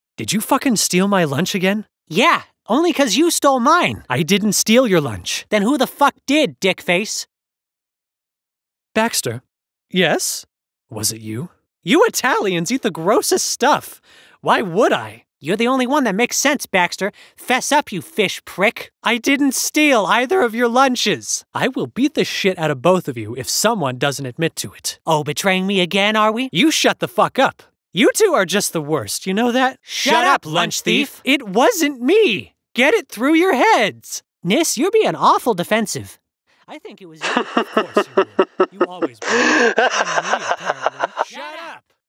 Lunch thief
No, it was not my intention for my last video to be about dinner and the very next one to be about lunch. Legit coincidence, occurred to me while editing lmao.

Arackniss, Angel Dust, Baxter & Alastor © Spindlehorse Toons/A24